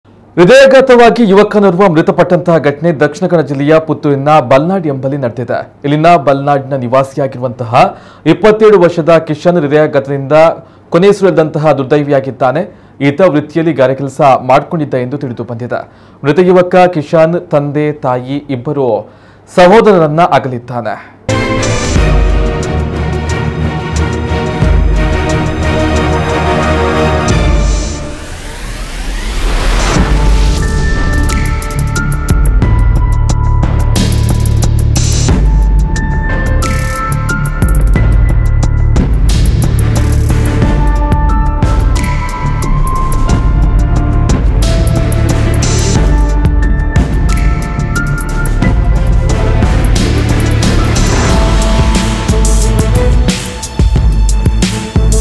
ಪುತ್ತೂರಿನಲ್ಲಿ 27 ವರ್ಷದ ಯುವಕ ಹೃದಯಘಾತಕ್ಕೆ ಕೊನೆಯುಸಿರು…!
ಹೃದಯಘಾತವಾಗಿ ಯುವಕನೋರ್ವ ಮೃತಪಟ್ಟ ಘಟನೆ ದಕ್ಷಿಣ ಕನ್ನಡ ಜಿಲ್ಲೆಯ ಪುತ್ತೂರಿನ  ಬಲ್ನಾಡ್  ಎಂಬಲ್ಲಿ ನಡೆದಿದೆ.
ತಾಜಾ ಸುದ್ದಿಗಳಿಗೆ  ನಮ್ಮ ಕುಡ್ಲ whatapp ಗ್ರೂಪ್‌ಗೆ  ಸೇರಿ..